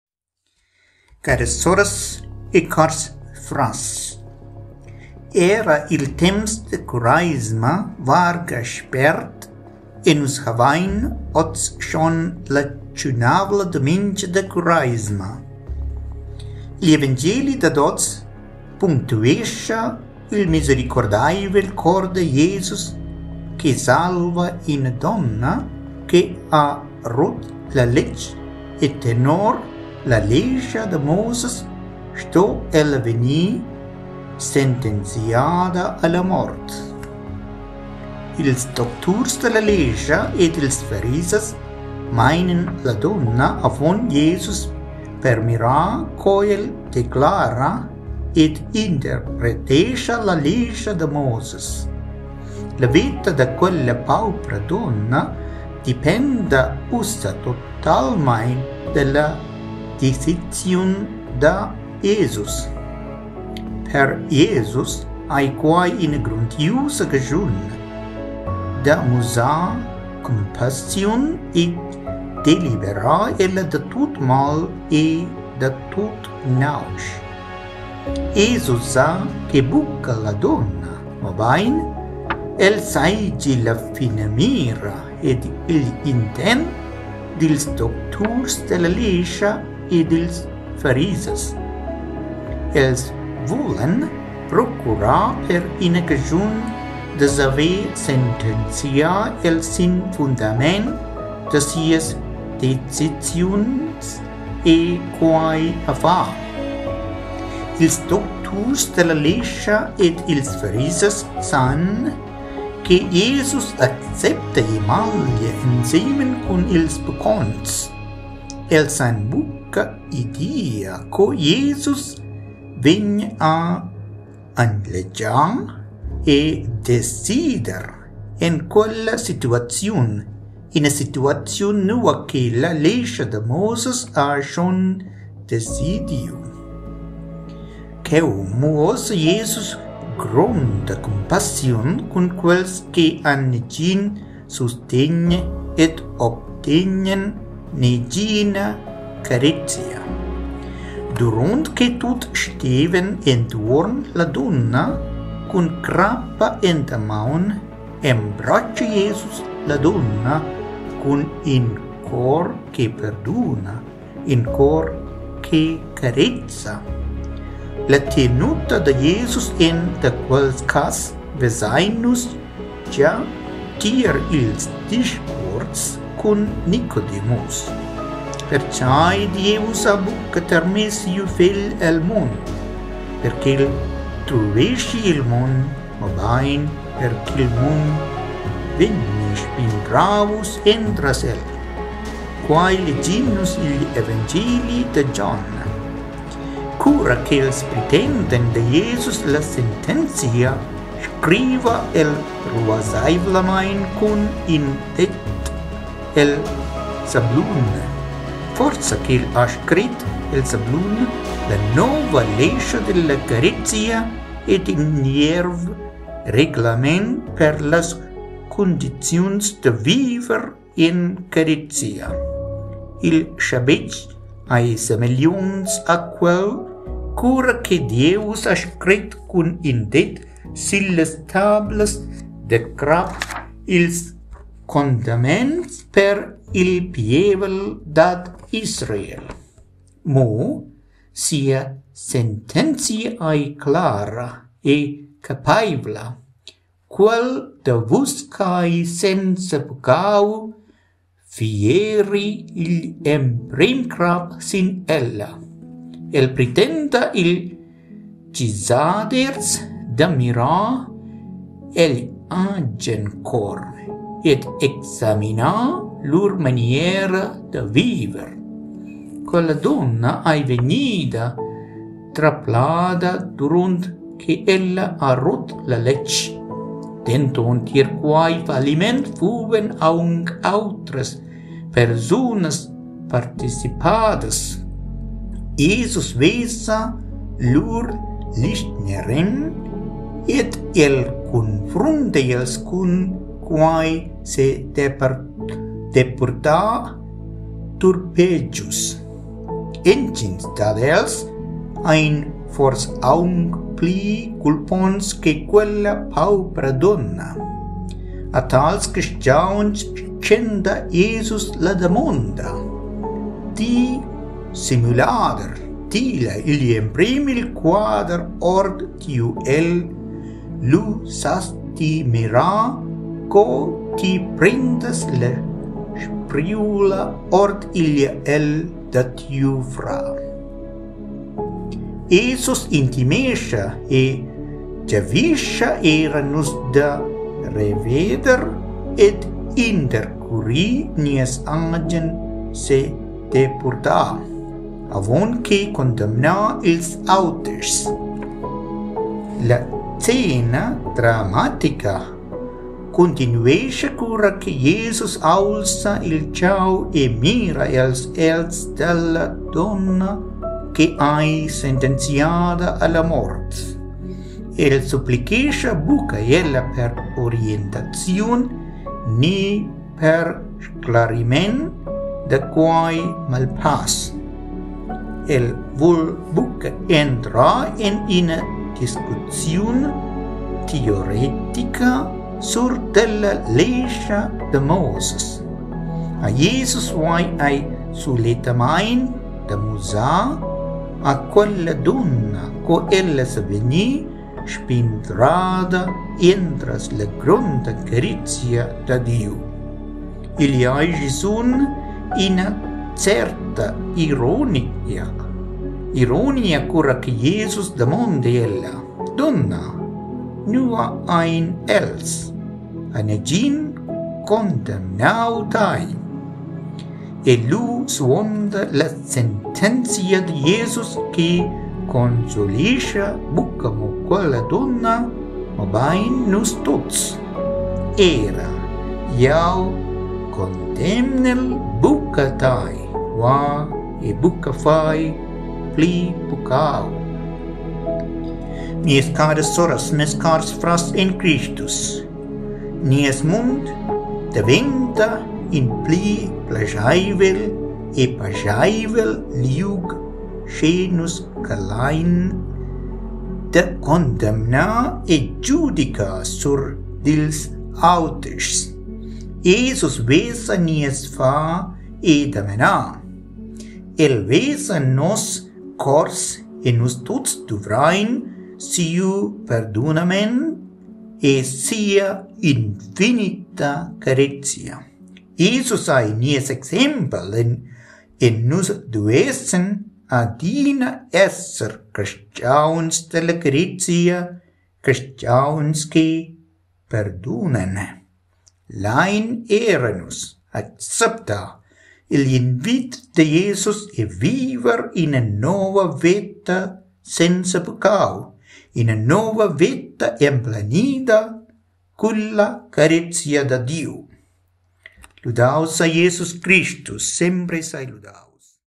La tschunavla dumengia da Cureisma ( Gion 8:1-11 ), 3 d’avrel 2022 | Homily in Romansh
Homily for the 5th  Sunday of Lent Year C, 3rd April 2022 | John 8:1-11
By Fr Matthew Charthakuzhiyil

Homily in Romansh